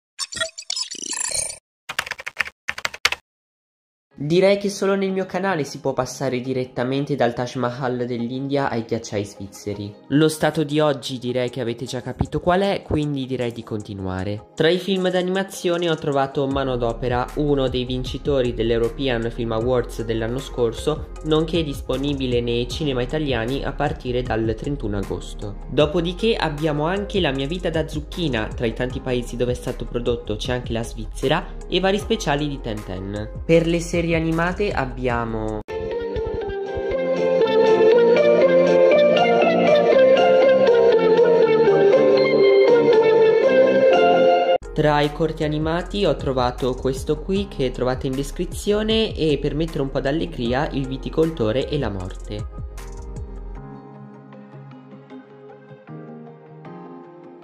Svizzera: film d'animazione creati in un determinato stato
Ciao gente! Se non sentite l'audio del video, scrivete nei commenti!!! Yt oggi è andato in palla e alcuni di voi potrebbero non sentire una cippa di quello che ho detto per ragioni ignote che il social non ha il piacere di rivelarci. Per gli stati da propormi scrivete nei commenti. Cercherò di accontentarvi tutti!!! Seguitemi anche su Instagram dove pubblico molte cose interessanti.